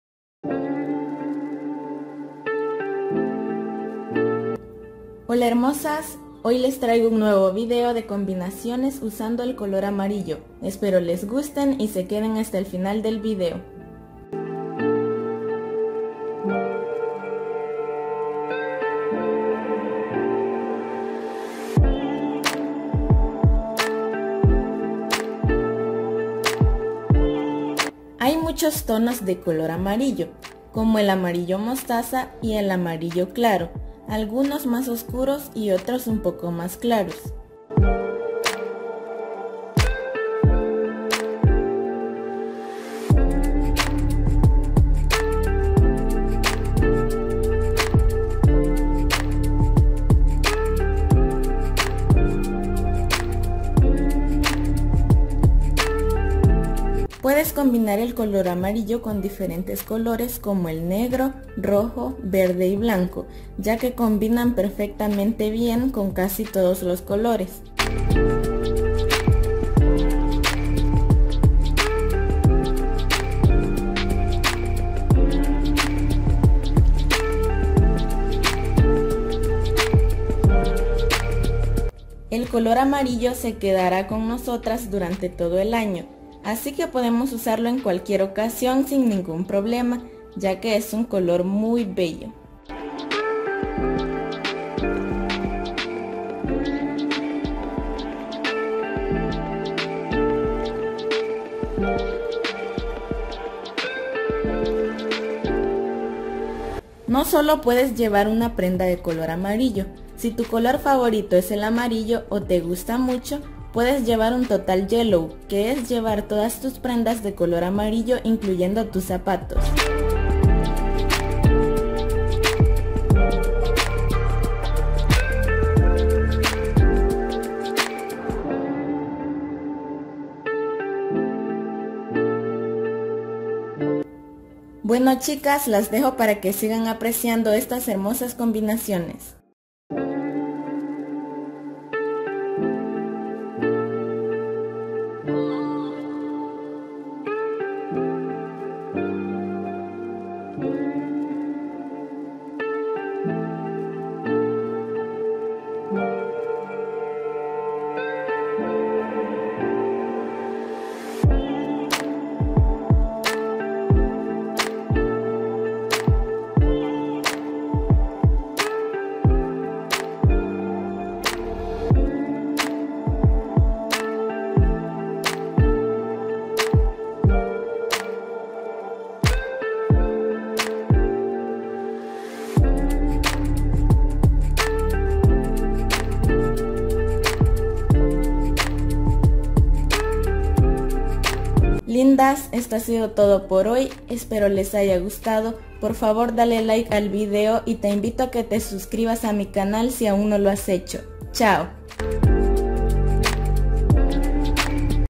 HERMOSAS COMBINACIONES USANDO EL COLOR AMARILLO DE MODA 2021/IDEAS PARA COMBINAR EL COLOR AMARILLO
Mis hermosas aquí les dejo el nuevo vídeo de combinaciones usando el color amarillo!

Les cuento que el color amarillo será la tendencia durante este año 2021, así que aprovechemos a lucirlo en cualquier ocasión!

Si te encanta este color puedes lucirlo por completo con un TOTAL YELLOW y verte muy espectacular a donde quiera que vayas.